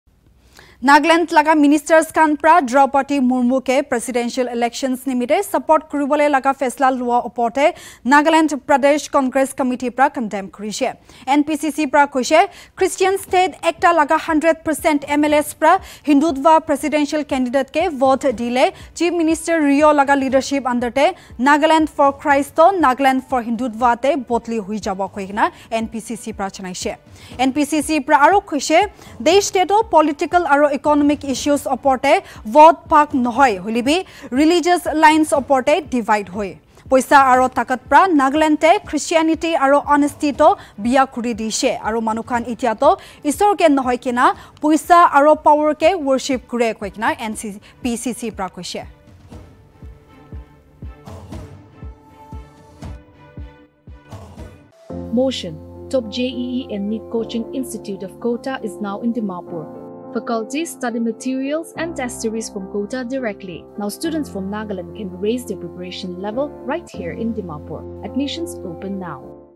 NPCC SLAMS UDA’S DECISION TO SUPPORT DRAUPADI MURMU
Coming strongly against Nagaland Legislative Assembly members' decision to support NDA candidate Draupadi Murmu in the upcoming presidential election, the Nagaland Pradesh Congress Committee on Saturday July 16, issued a press release condemning the move of the UDA coalition govt. The NPCC mentioned that mlas of a christian state under the leadership of CM Neiphiu Rio voting for hindutva presidential candidate will transform Nagaland for christ into Nagaland for hindutva. NPCC further stated that political and economic issues in India are divided on religious lines. NPCC furthermore lamented that money and power have destroyed honesty and christianity in Nagaland and people nowadays worship money and power and not god